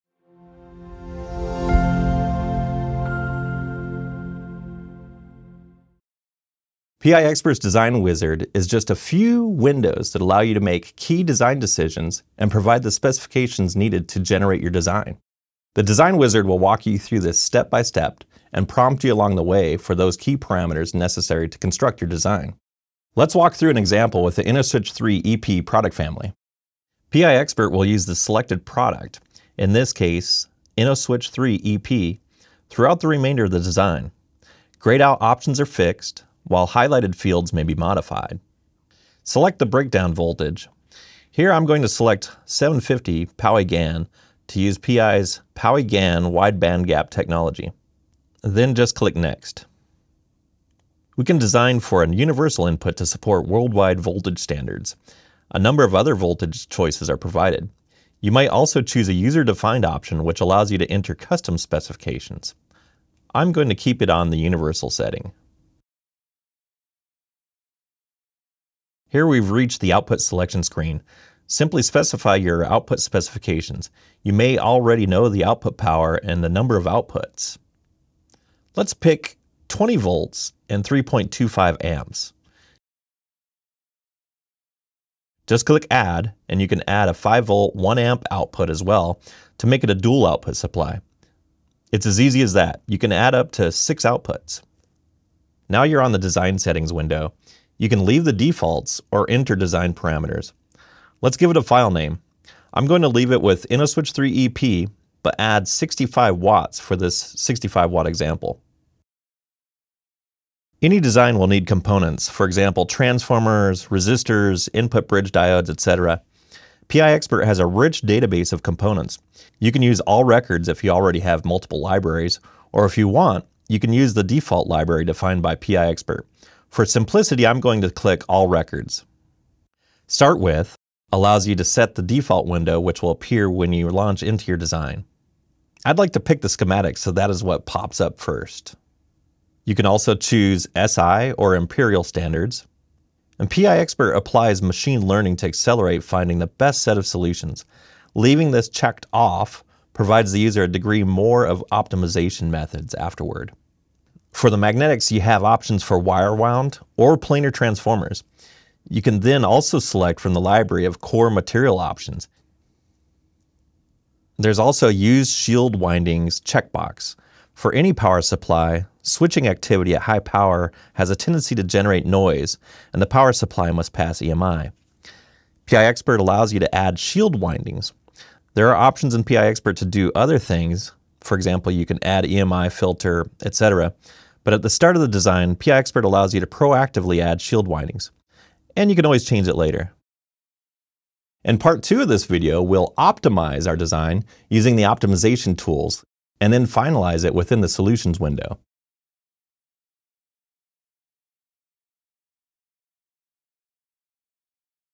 PI Expert - Design Wizard Takes You from Specifications to Complete Designs, Step by Step
PI Expert features a Design Wizard that can walk you through the design process step-by-step. All you need to do is to provide key perimeters when prompted along the way. Watch Director of Channel Marketing Trevor Hiatt demonstrate how to create a easy-to-build power supply design within minutes.